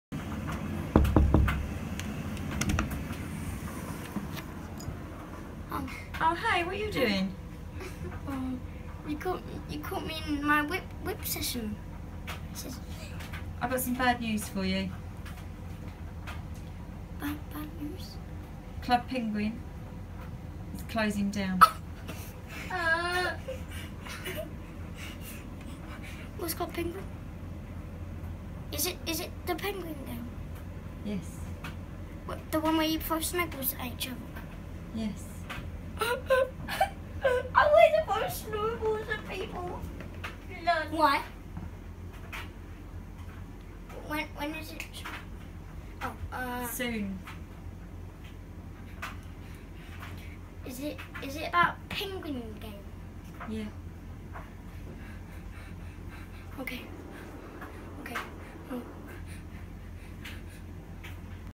Kid gets mad over club penguin shutting down!
This kid gets mad over club penguin and is so funny to watch!